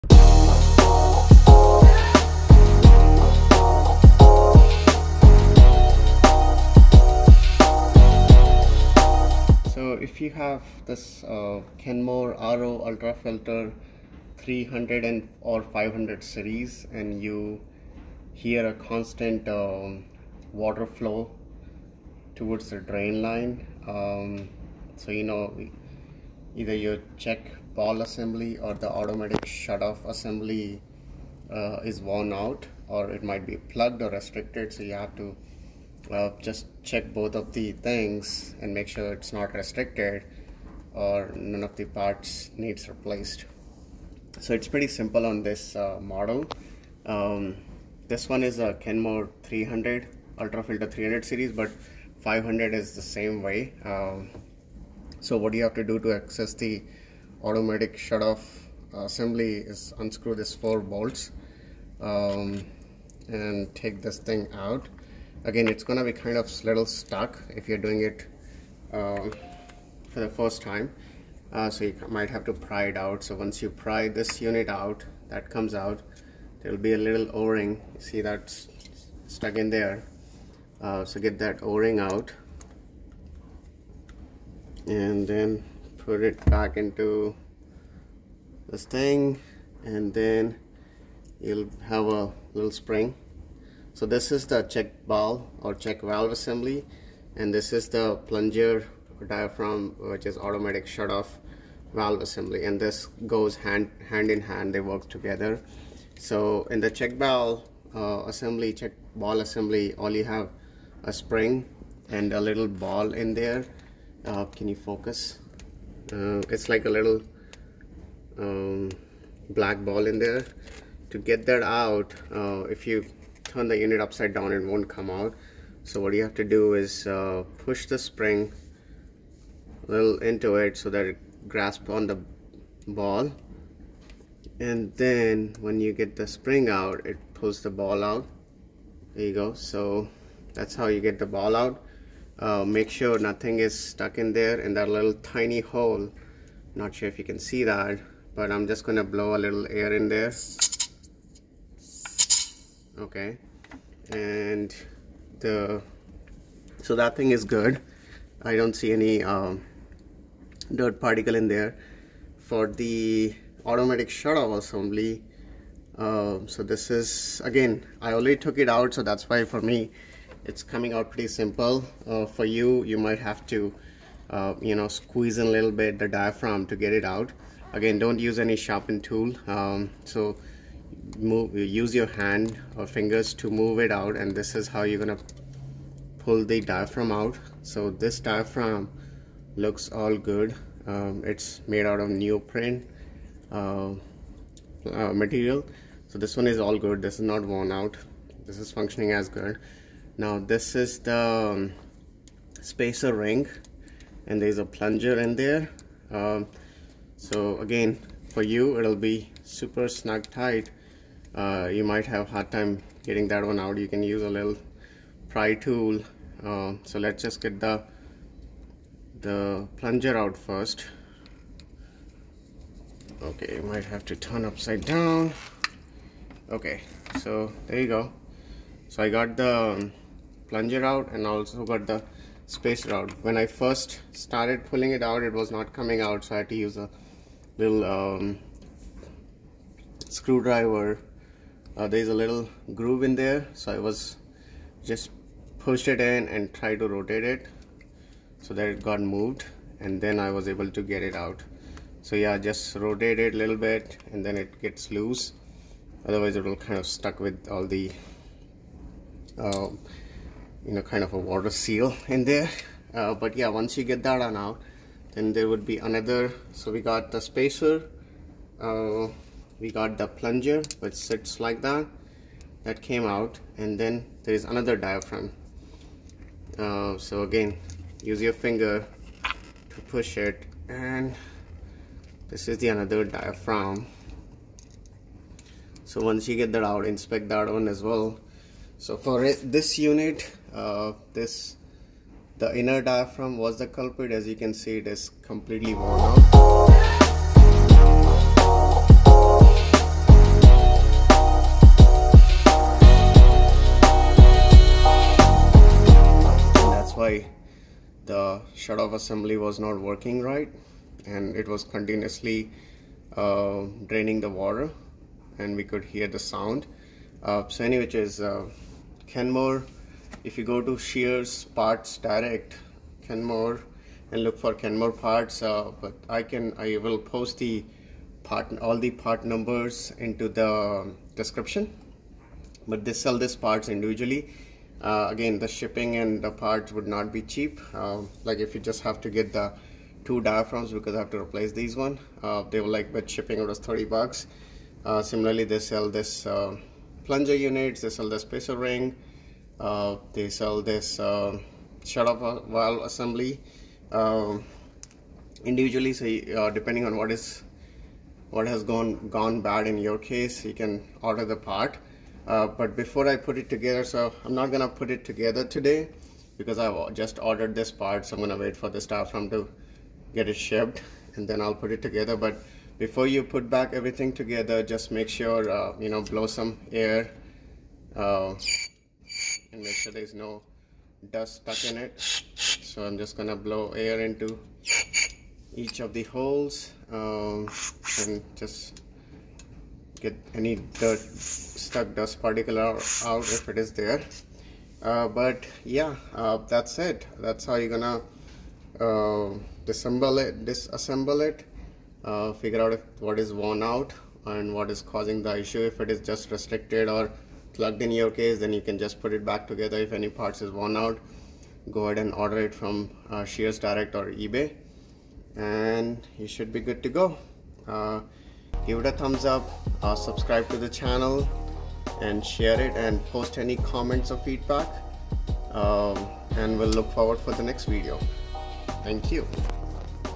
Troubleshooting Reverse Osmosis System Constant Draining - Sears Kenmore RO Ultrafilter
If your RO Reverse Osmosis System is continuously draining/ your hear constant water running noise / Continual water flow to drain, then it means the system is either not detecting the reservoir tank is completely full Or not able to shut-off the water supply.

Reservoir full tank pressure is detected by Check Valve/Ball assembly, so it might be plugged, restricted or parts are worn.

Water supply shut-off is controlled by Automatic Shut-off assembly, so again failure is caused by plugged, restricted, or worn parts.

The troubleshooting is performed on Kenmore RO Ultrafilter 300 model, but the same applies to Kenmore RO Ultrafilter 500 & 550 model.

Sears Kenmore Part#:
Diaphragm Kit - 7250876‬
Plunger & Spacer Ring Kit - 7234325‬
Check Ball/Valve Assembly - 7234317

to buy the parts if not found on eBay.

Likewise, the troubleshooting process is more or less the same with other RO systems:

❇️ Follow this video, and fix your RO System Continual water draining issue.

All feedback & questions are welcomed. 🏳️‍🌈